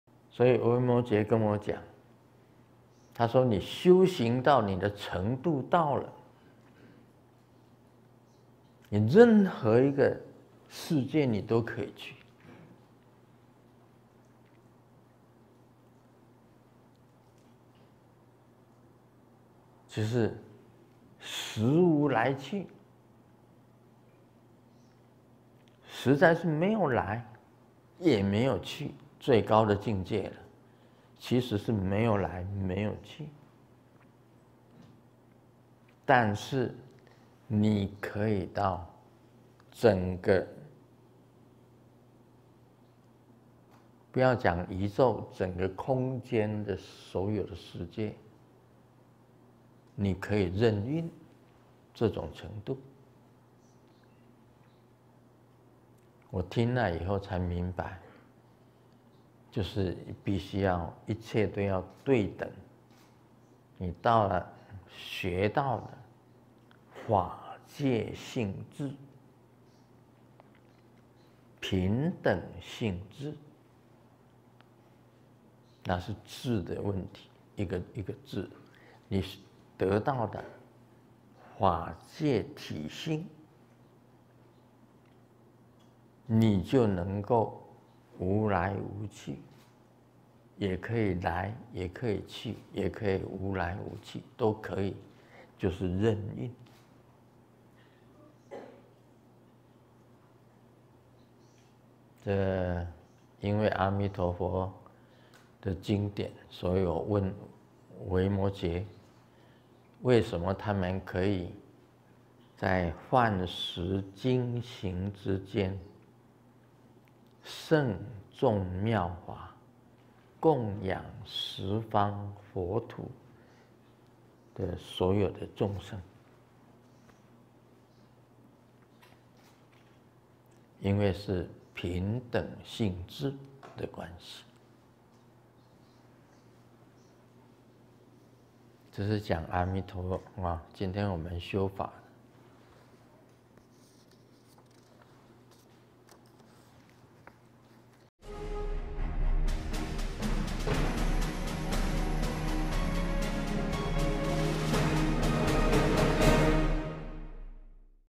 20220917聖尊蓮生活佛盧勝彥開示：修行程度到了，任何世界都可以去，實無往來－TBSN高清字幕版
20220917聖尊蓮生活佛盧勝彥開示：修行程度到了，任何世界都可以去，實無往來－TBSN高清字幕版